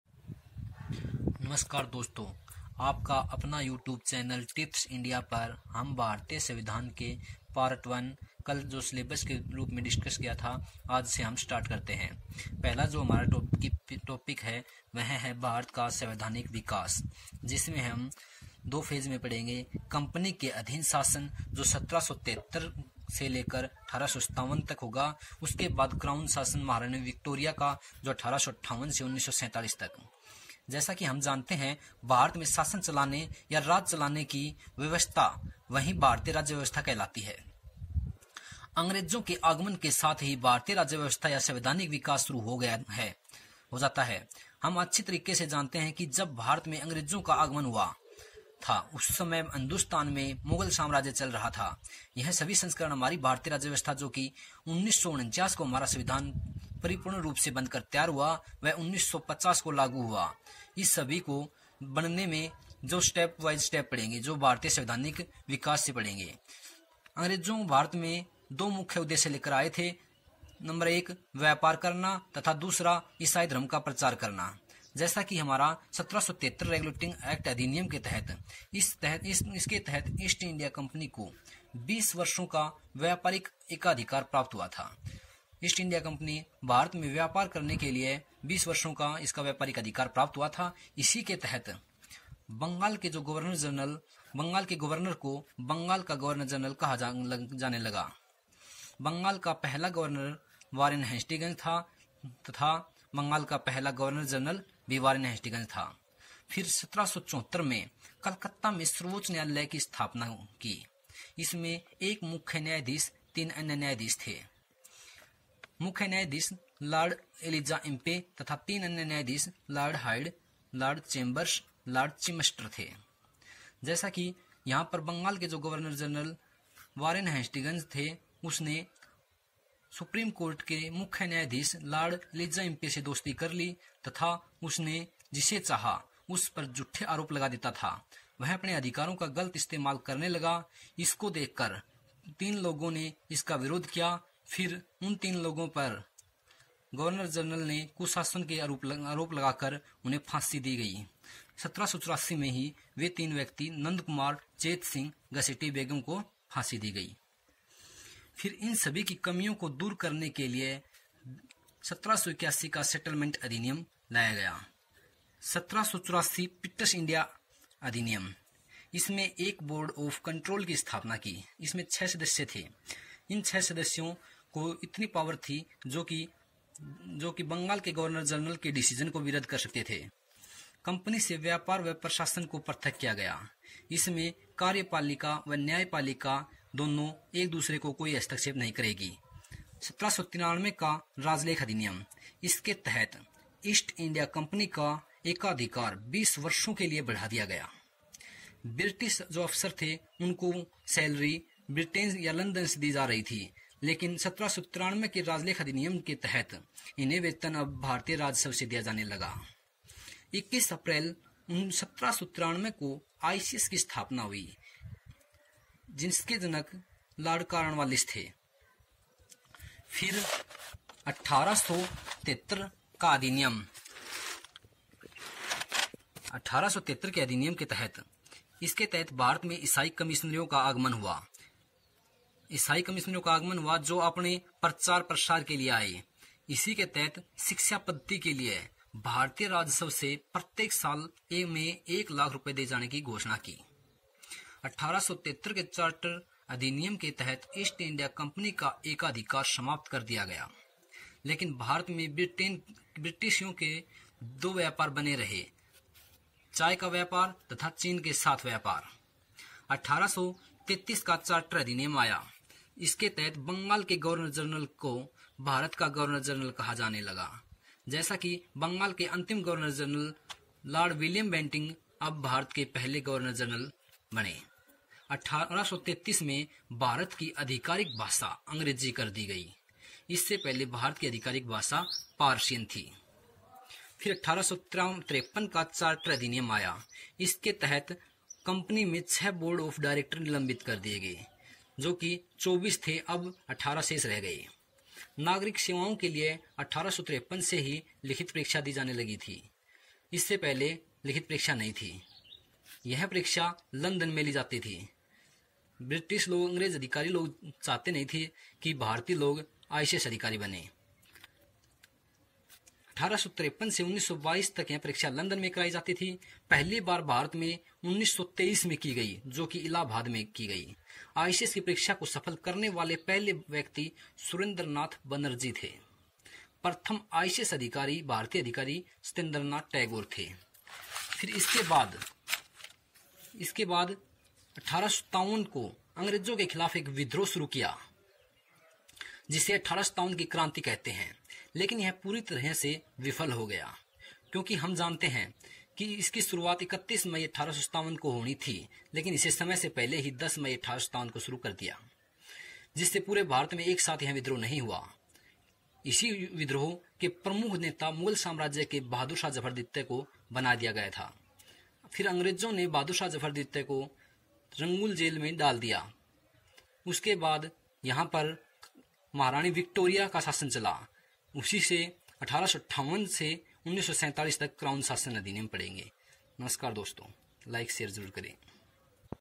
Lecture-1 || origin of Constitution during company rule(1773-1858) || Indian polity
For Hssc, ssc, Ntpc, Delhi Police👮🚓, Upsc etc